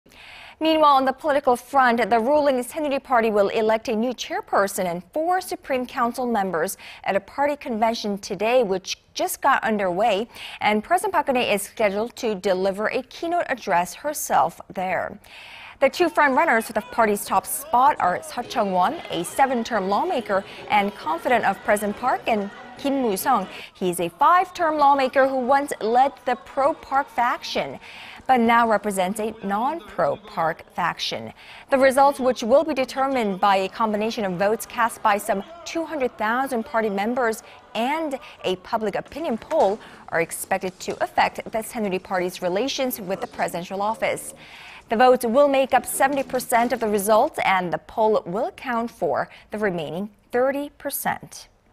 Ruling Saenuri Party to elect new leadership at convention Monday
The ruling Saenuri Party will elect a new chairperson and four Supreme Council members at a party convention today,... and President Park Geun-hye is scheduled to deliver a keynote address there.
The two frontrunners for the party's top spot are Suh Chung-won , a seven-term lawmaker and confidant of President Park,... and Kim Moo-sung, a five-term lawmaker who once led the pro-Park faction... but now represents a non-pro Park faction.
The results, which will be determined by a combination of votes cast by some 200-thousand party members and a public opinion poll, are expected to affect the Saenuri Party's relations with the presidential office.
The votes will make up 70 percent of the result and the poll will count for the remaining 30 percent.